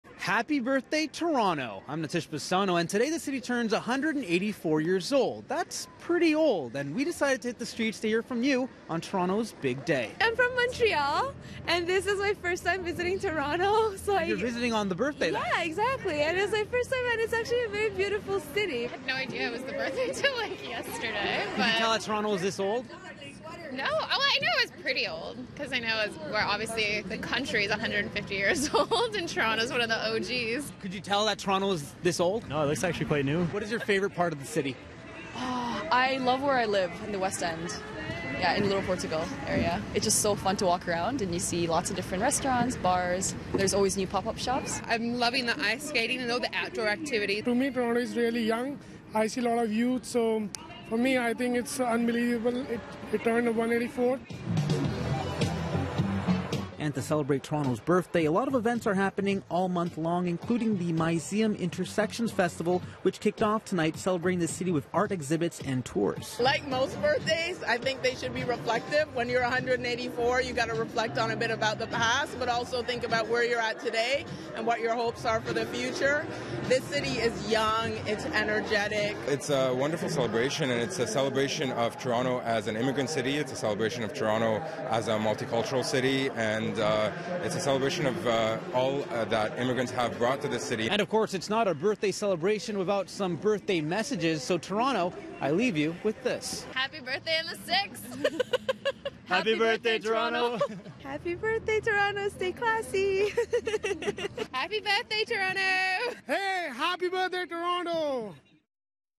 Celebrating Toronto’s 184th birthday
Nitish Bissonauth speaks with Toronto locals and visitors to find out how they’re celebrating the city’s 184th birthday.